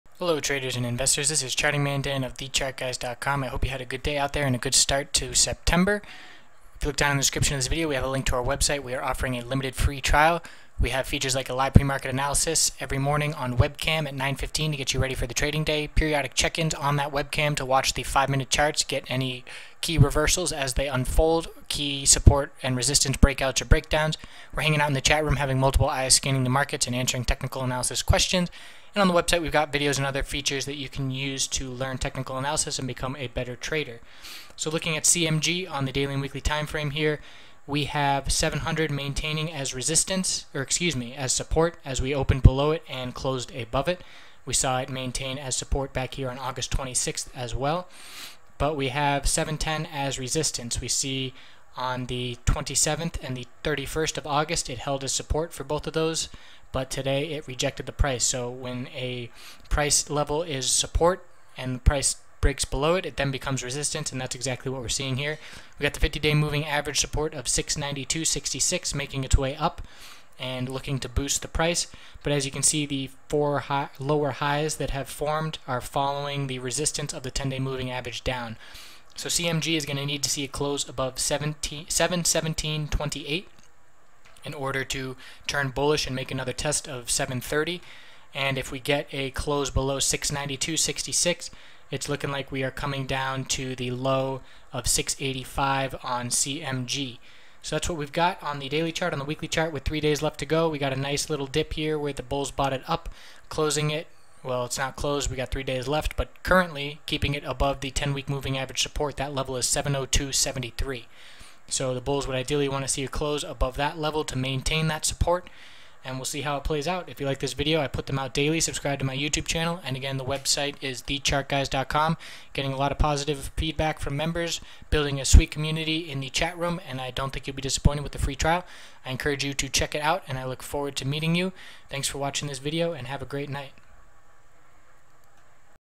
CMG Technical Analysis Video 9/1/2015
A look at the daily and weekly CMG charts. Questions, comments welcome.
Checkout the free chat while we get the website started.